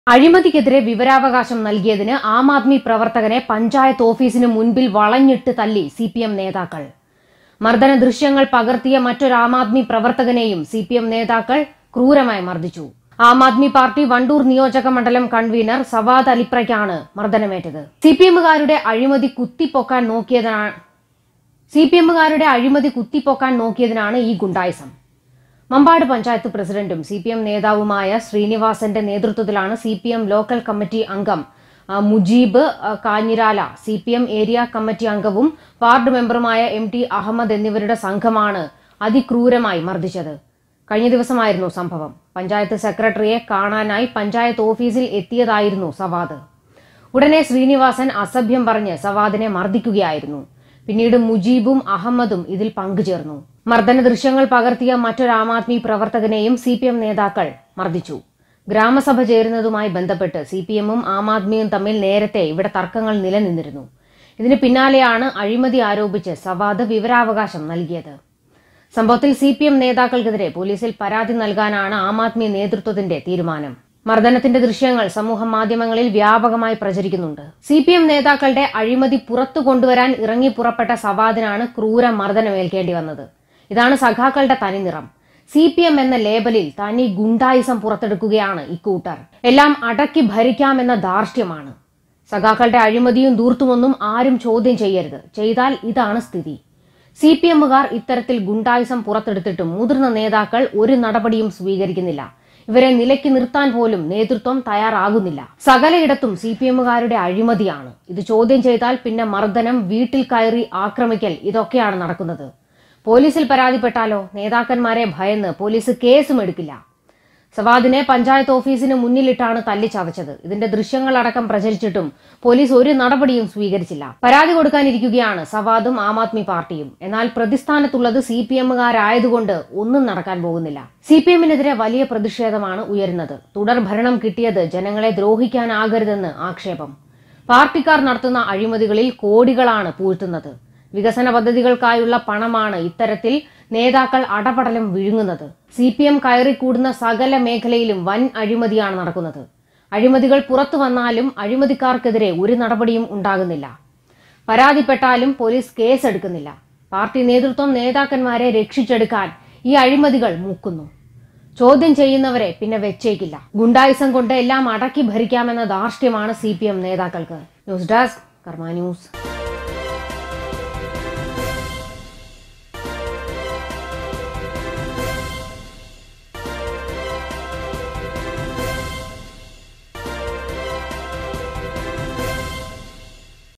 AAP പ്രവര്‍ത്തകനെ വളഞ്ഞിട്ട് ആക്രമിച്ച് CPM നേതാക്കള്‍
CPM നേതാക്കന്മാരുടെ അഴിമതി പൊക്കാന്‍ നോക്കി,AAP പ്രവര്‍ത്തകനെ പഞ്ചായത്ത് ഓഫീസിന് മുന്‍പില്‍ വളഞ്ഞിട്ട് തല്ലി CPM നേതാക്കള്‍